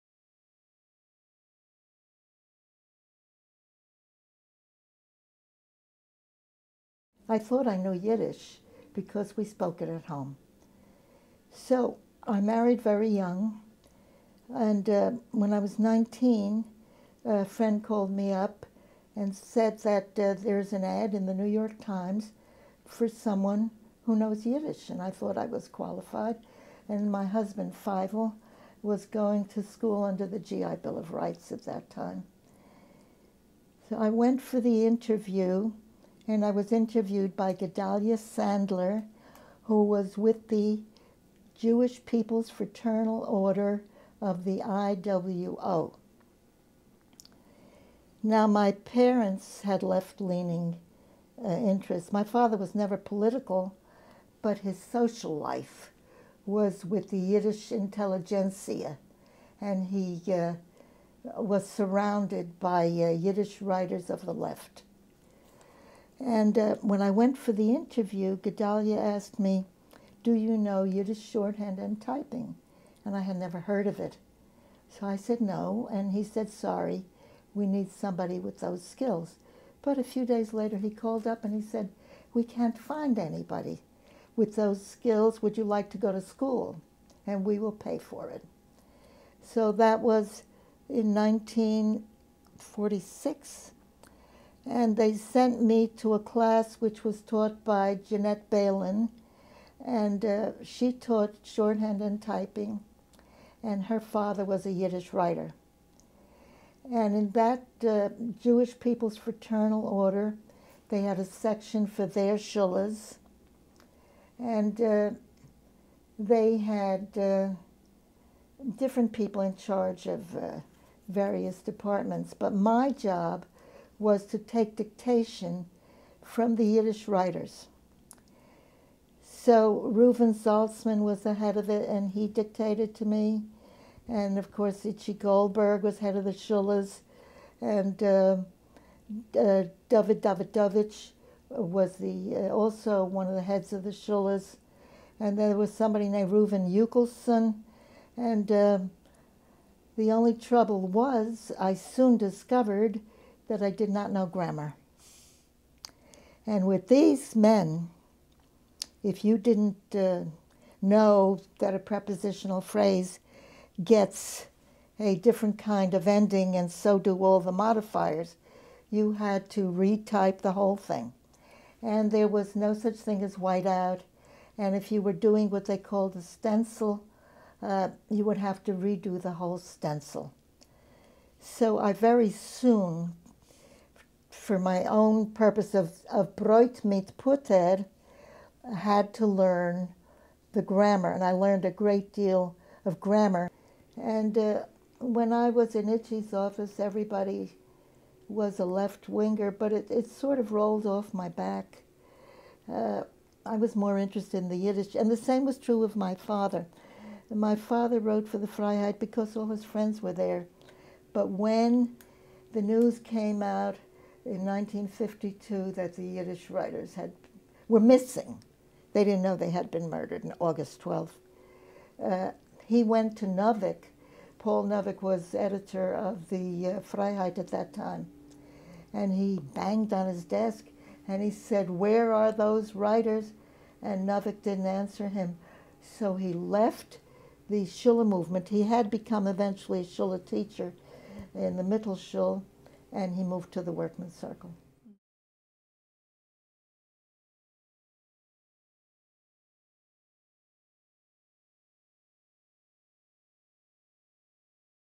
The JPFOIWO: The World of the Yiddish Intelligentsia
Troim Katz Handler, daughter of Yiddish poet Menke Katz, describes the secretarial position that she held for many years for the Jewish People's Fraternal Order of the IWO, a secular Jewish, Communist organization. She recalls that neither she nor her father were particularly politically inclined, and Menke in fact left the Freiheit after the murder of Jewish writers by Stalin in 1952.